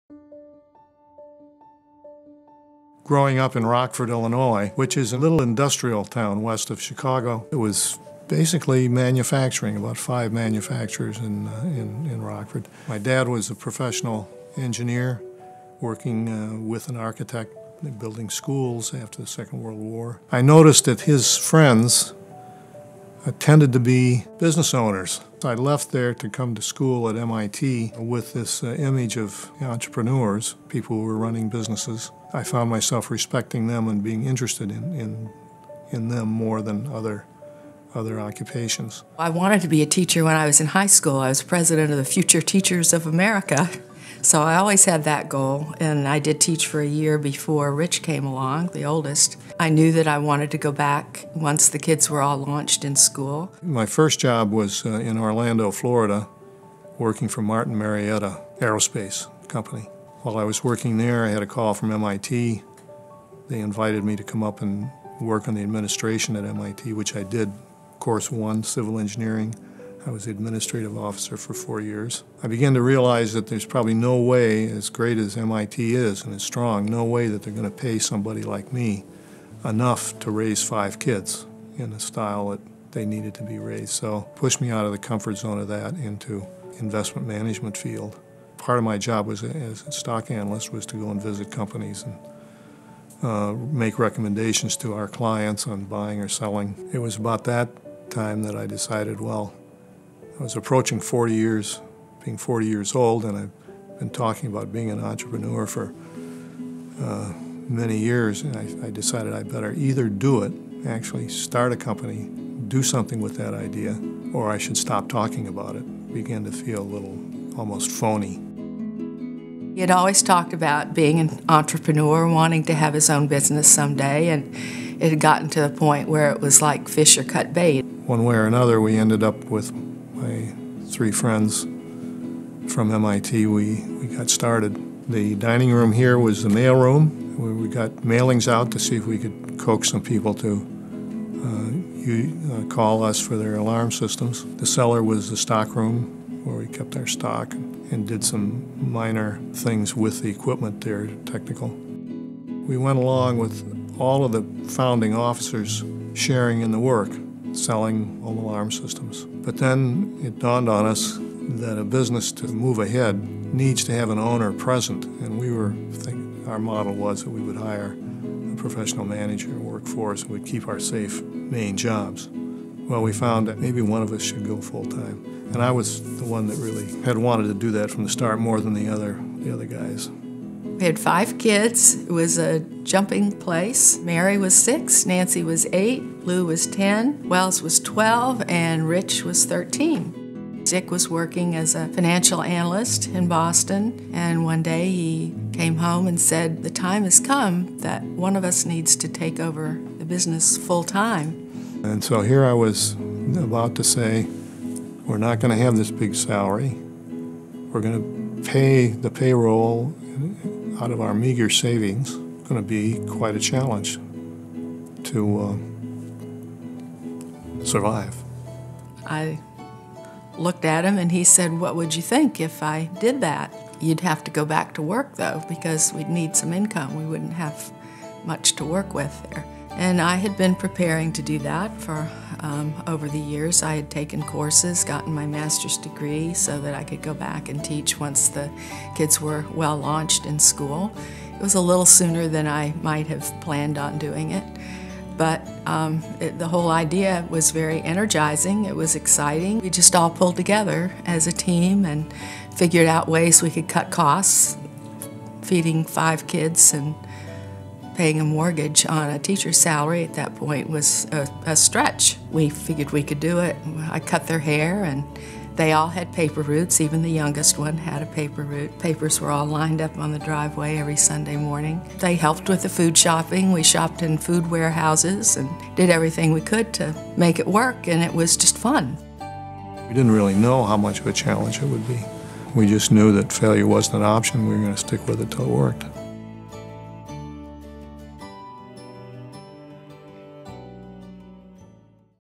American Alarm -Teamwork from the Start
An interview with Dick and Betsy Sampson on the early days of American Alarm and how the whole Sampson family pulled together to make it work. It's that sense of teamwork that has grown with the company through the years and helped American Alarm be the company it is today.